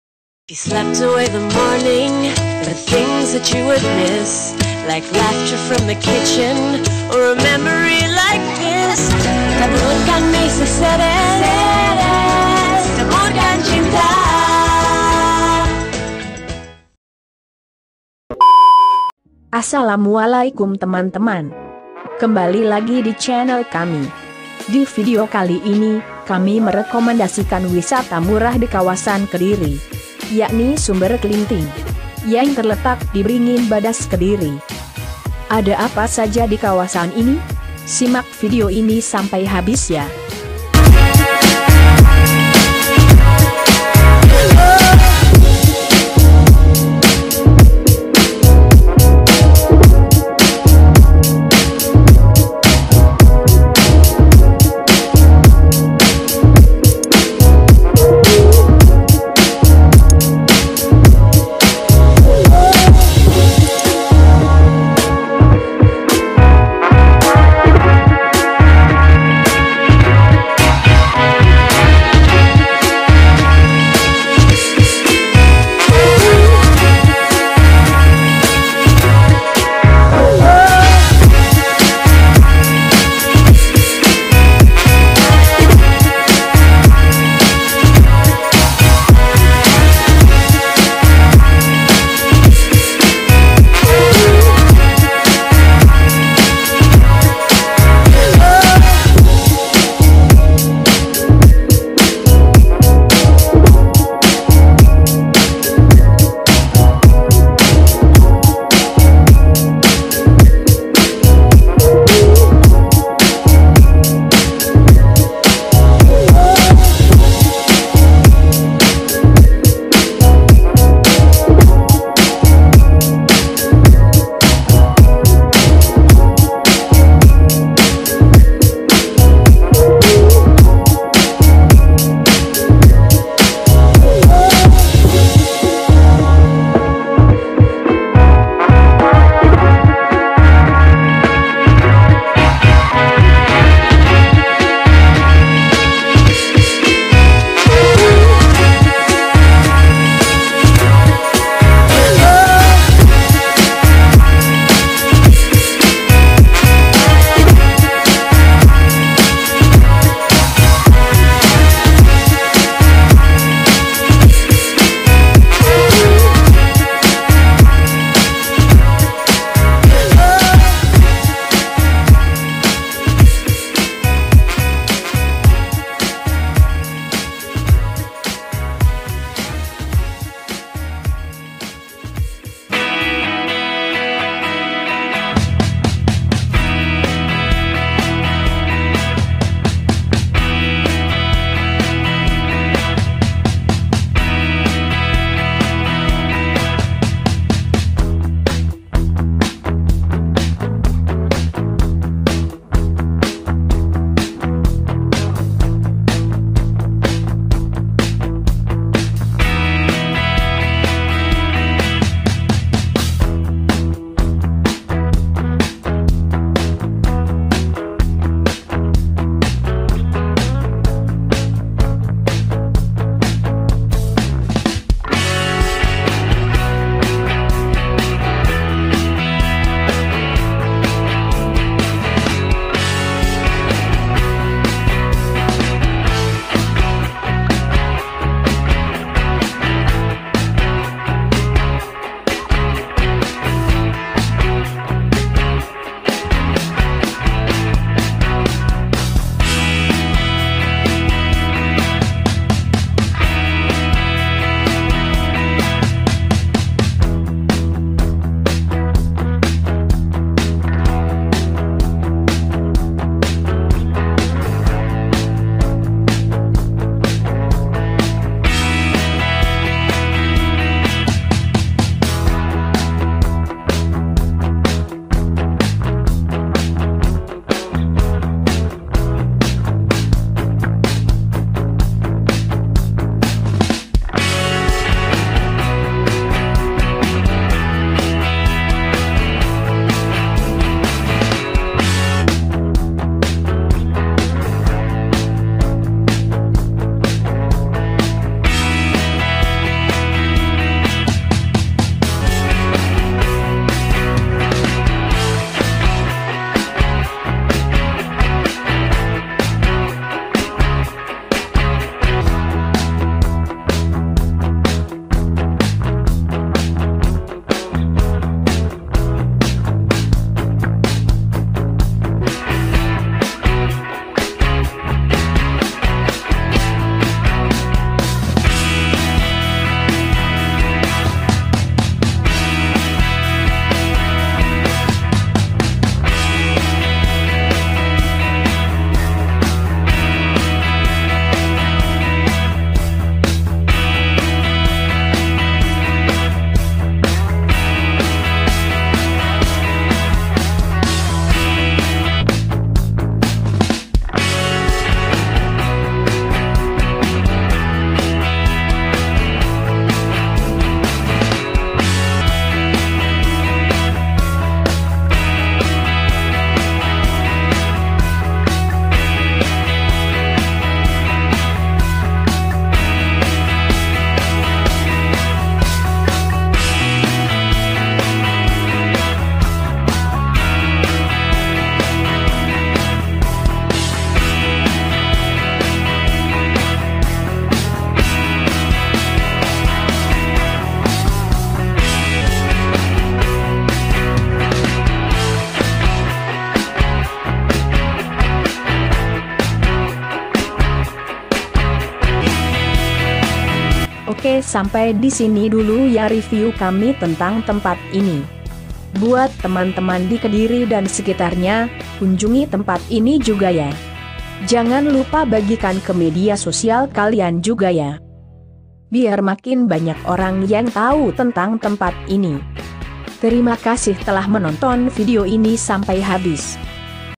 Pesona Wisata Desa "SUMBER KLINTING" Rekomendasi Wisata Murah Kediri
Assalamualaikum,
Video kali ini kami akan merekomendasikan Wisata Murah di Kediri yang bernuansa alam pedesaan yakni Sumber Klinting, berlokasi di Bringin Badas kab. Kediri,
HTM Dewasa 2ribu, Anak" Gratis, Parkir Gratis,
Untuk informasi selengkapnya silahkan simak video ini selengkapnya,,,